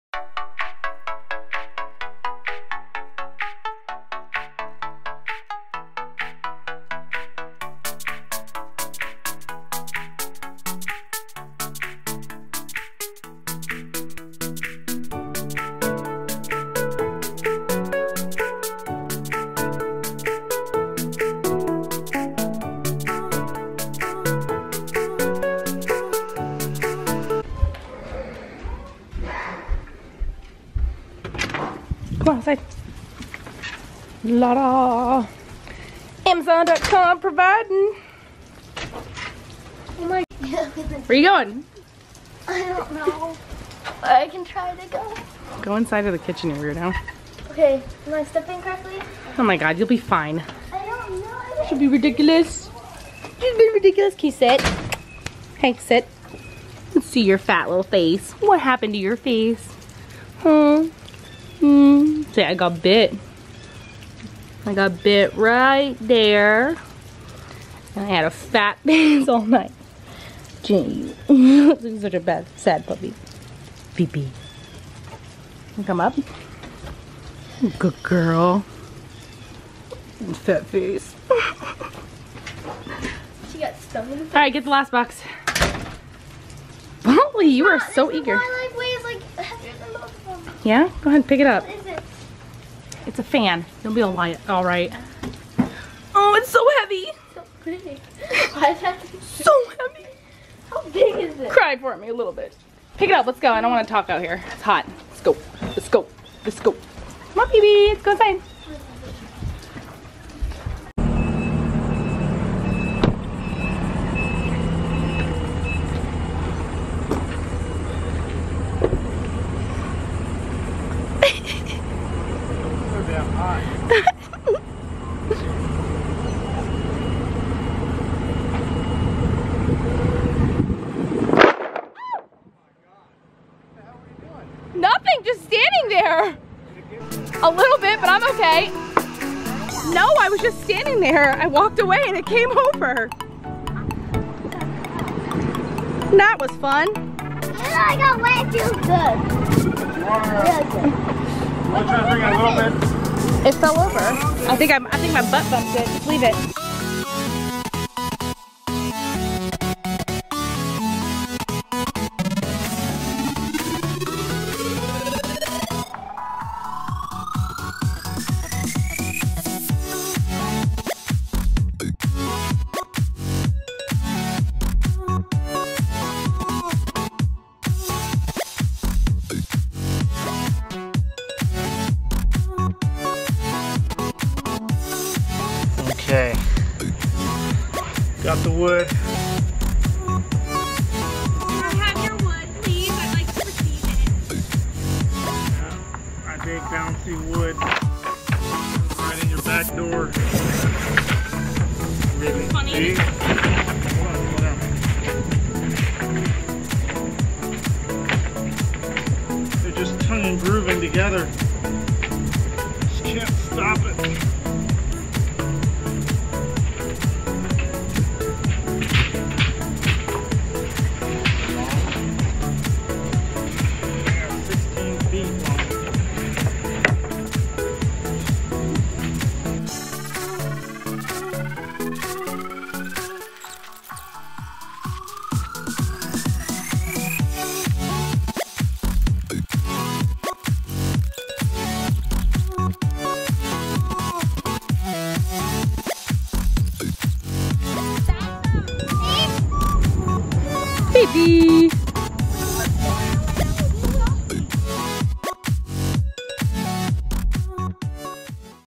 9. Our Skoolie Conversion | Off To Get Our Ceilings
thebhive family is off to get our ceiling materials from NC Lumber & Supply.  We chose to use 4 in Pine Tounge and Groove. Now to get them up on the ceilings

Check out NC Lumber & Supply if you are in the Wilmington area, great prices!